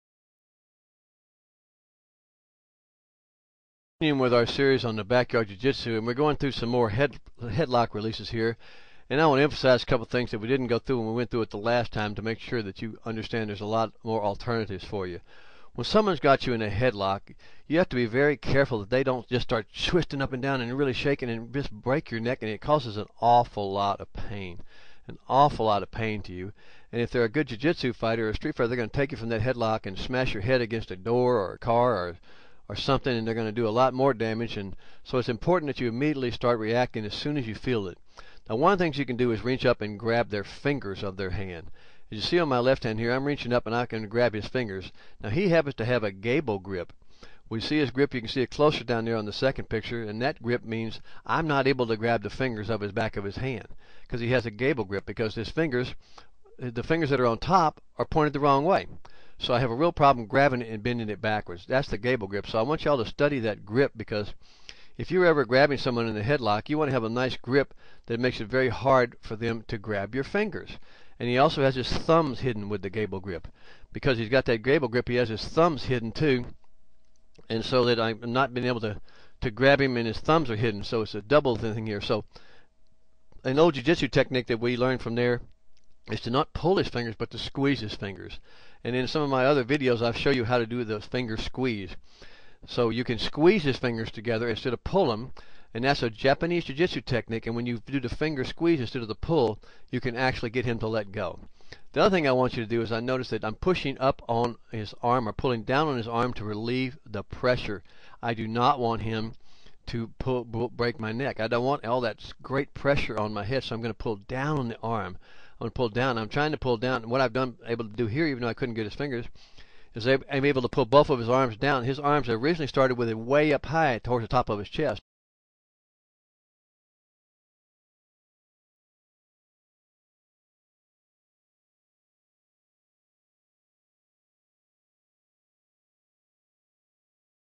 Backyard Jiu Jitsu, part 6
Continue your backyard jiu jitsu training with learning how to escape head locks as well as bear hugs and wrist locks.